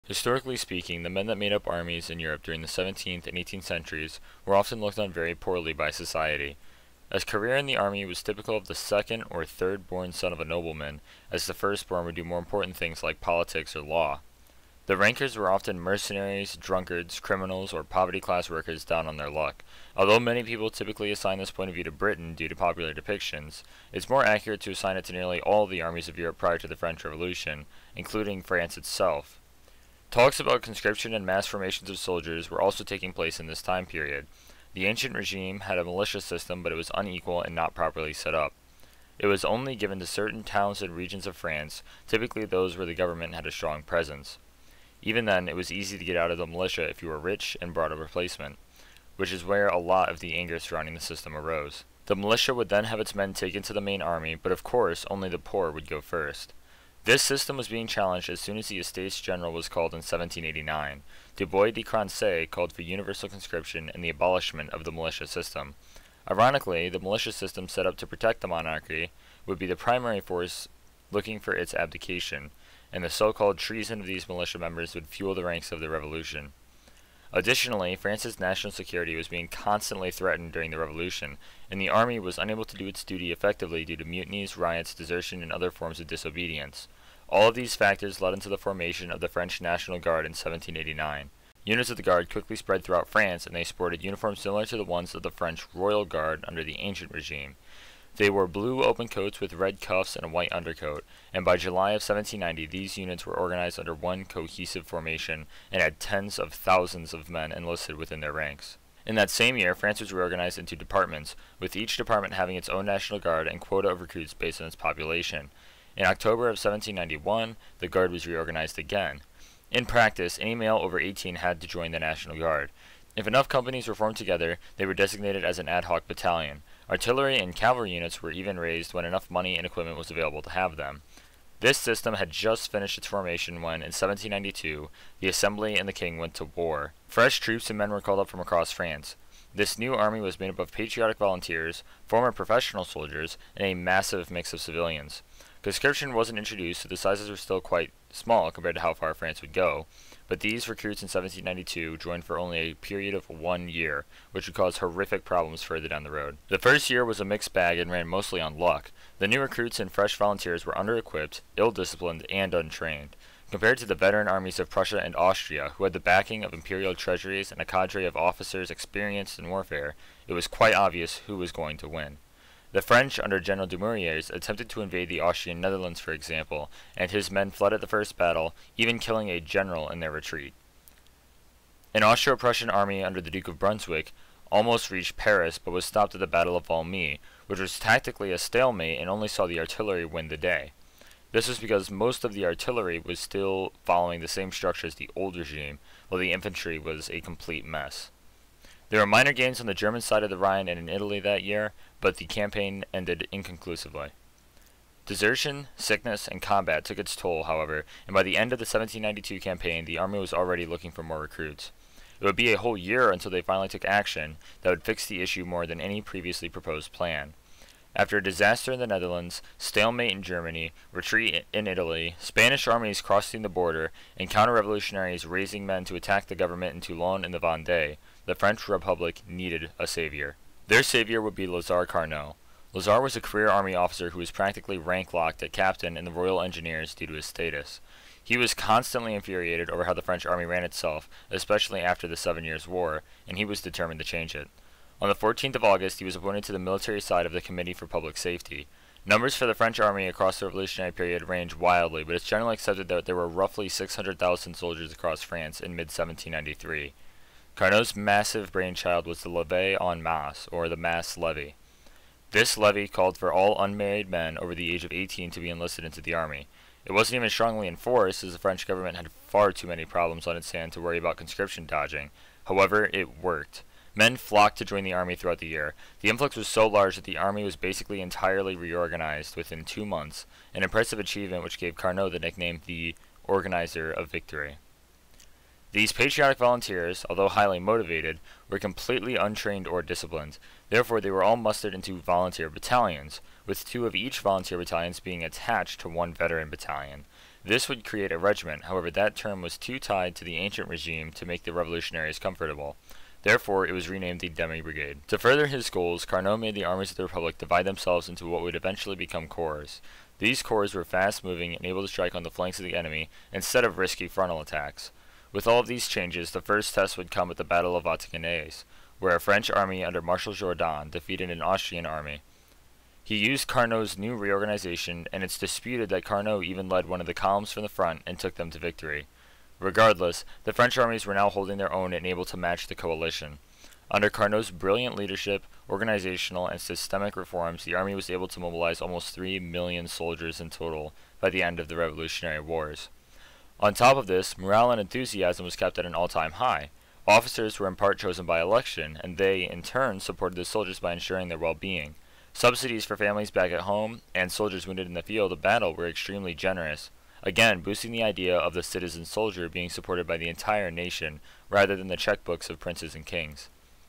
The Mobilization of France during the Revolutionary Wars
In 1789 France found itself on the beginning of a long trip through what's now called the Revolutionary Wars. In this video I try my best to cover the French mobilization efforts before and during the War of the First Coalition, specifically  centered around Army Reformer Lazare Carnot and the French National Guard, an institution surviving to this day.